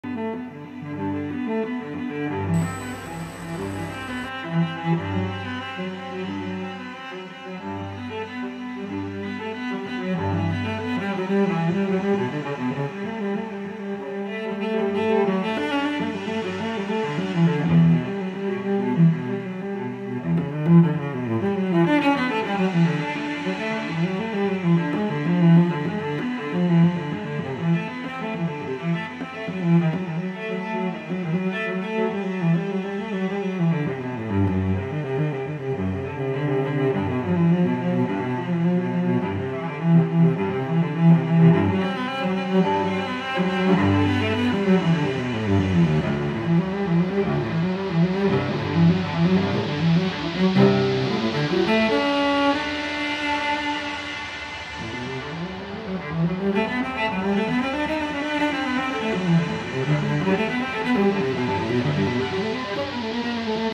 Bud Vase Turned from Scraps of Exotic Woods SD 480p
This vase was turned from leftover scraps of exotic woods that were glued together, re-sawed into cubes, and then sliced into layers to create movement of the colors across the vase.